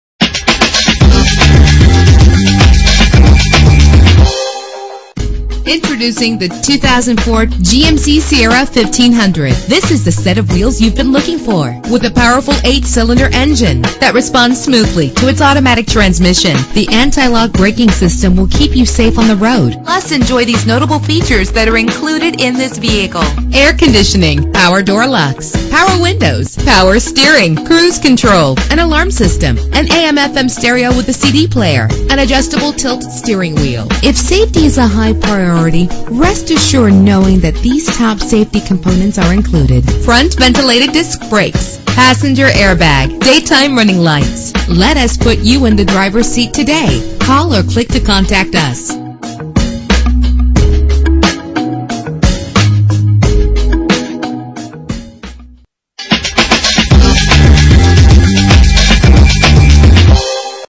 2004 GMC Sierra 1500 - Daphne AL
Year: 2004
Make: GMC
Model: Sierra 1500
Trim: SLE
Engine: 5.3 liter 8 cylinder 16 valve
Transmission: 4-Speed Automatic
Color: Summit White
Mileage: 63418
Address:
27161 Hwy 98